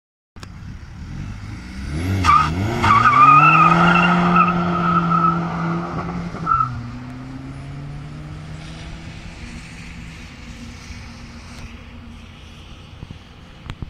VW Golf MK5 1.9 TDI 105hp GT1749v | Test Remap
►Hello everyone and welcome back. Today we show you a VW Golf MK5 1.9 TDI 105hp with Garret GT1749v From Seat Leon 1.9 150hp ARL Engine. Enjoy!

- GT1749V
- Inox Exhaust

Remap: FGPERFORMANCE

We are happy if you help us to grow and you allow us to making more videos like this and better.

VW Golf MK5 1.9 TDI 105hp GT1749v